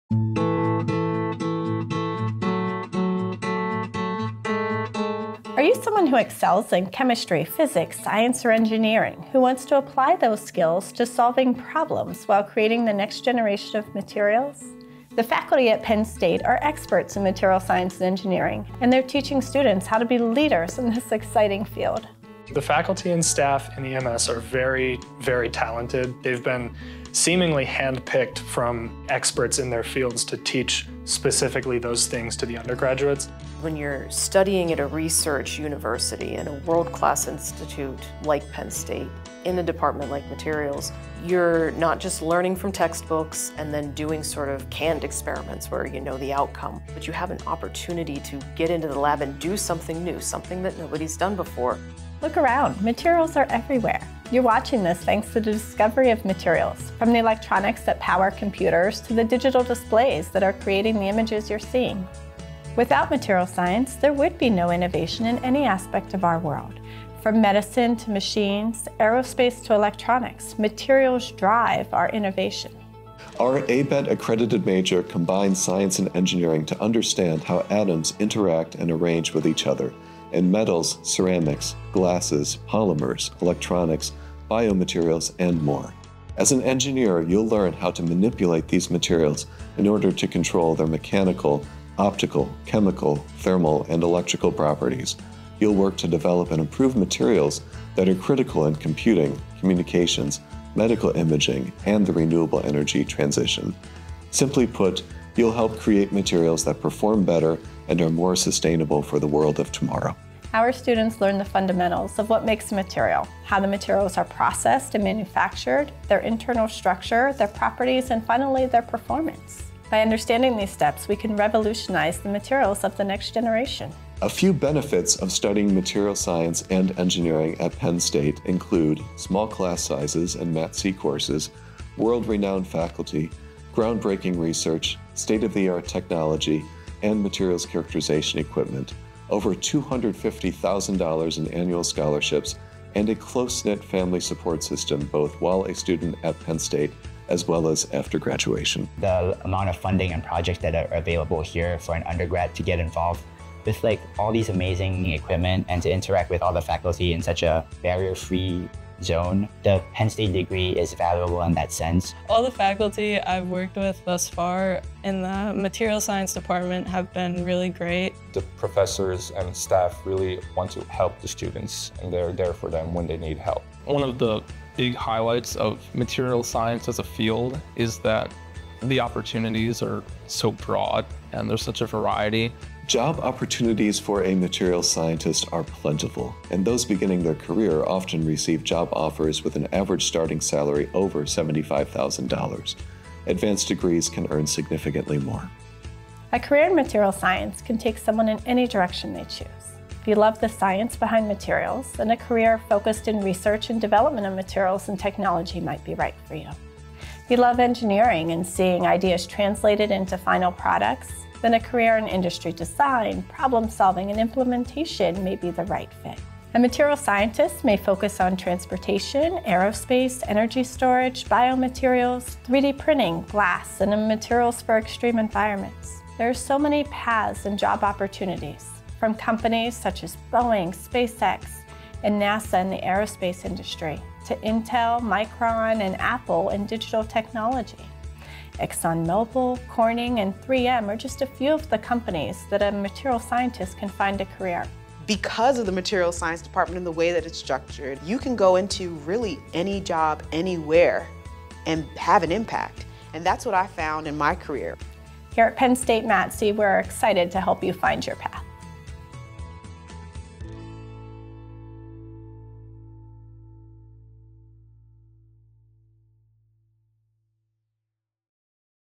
Explore your Future | Materials Science and Engineering
The Department of Materials Science and Engineering at Penn State is an international leader in materials education and research. As a top-ranked program, the department thrives on a rich collaboration between faculty, staff, students, and researchers to promote a well-rounded academic experience and innovative research opportunities. Our department offers ABET accredited degree programs at both the undergraduate and graduate levels.